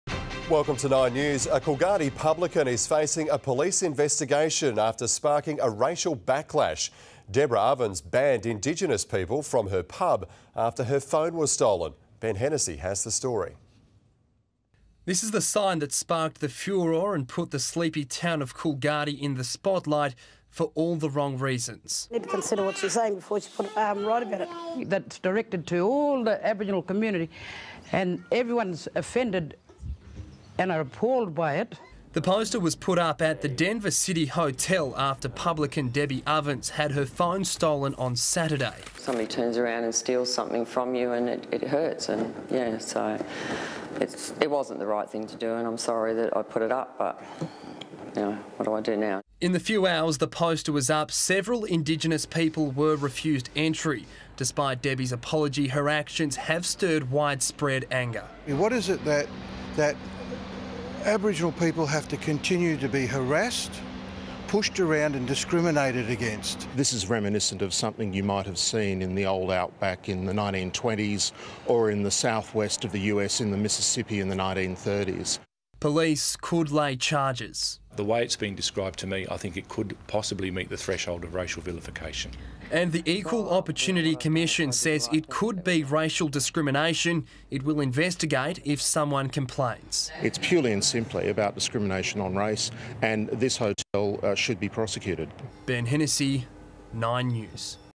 Pub ban backlash | 9 News Perth
A Coolgardie publican is facing a police investigation after sparking a racial backlash.
Deborah Ovens banned indigenous people from her hotel - after her phone was stolen.

Ben Hennessy has the story.